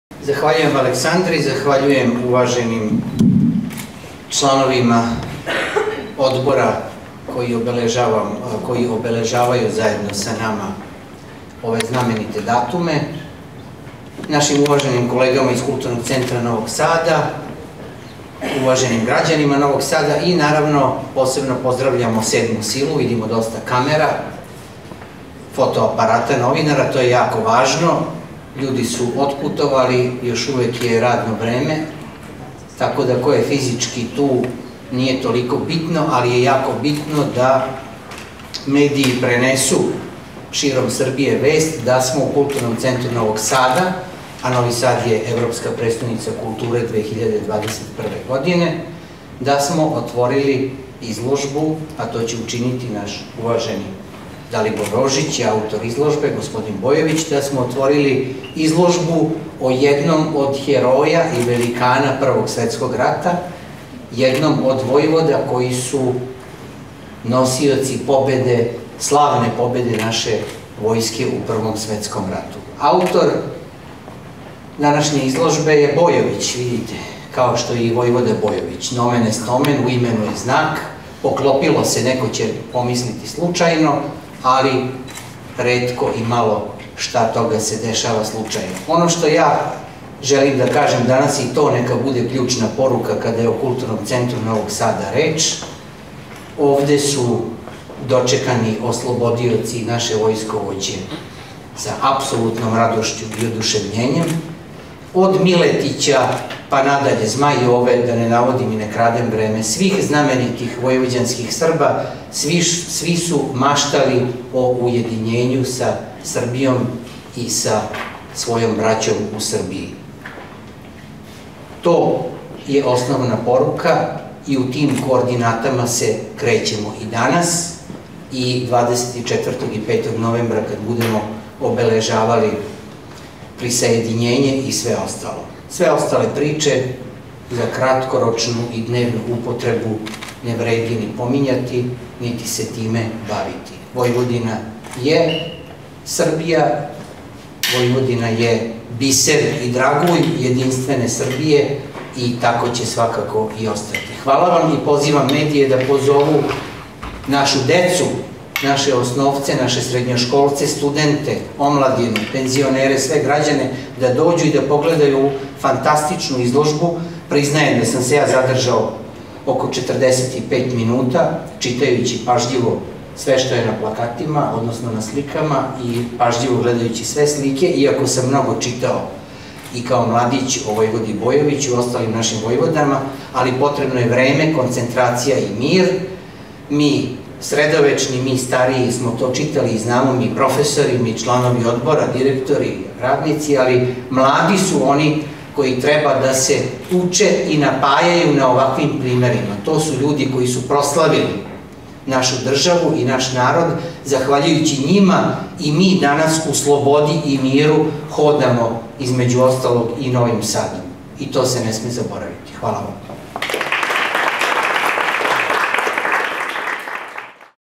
Бојан Панаотовић, директор КЦНС
У име Културног центра Новог Сада, директор Бојан Панаотовић казао је да је ово изложба посвећена војводи Петру Бојовићу, једном од српских великана Великог рата који су извојевали блиставу победу.
- У Новом Саду су ослободиоци и наше војсковође дочекани са одушевљењем свих знаменитих војвођанских Срба који су маштали о уједињењу војвођанских области са Краљевином Србијом – рекао је Панаотовић, уз констатацију да је Војводина данас део Србије, њен бисер и тако ће и остати.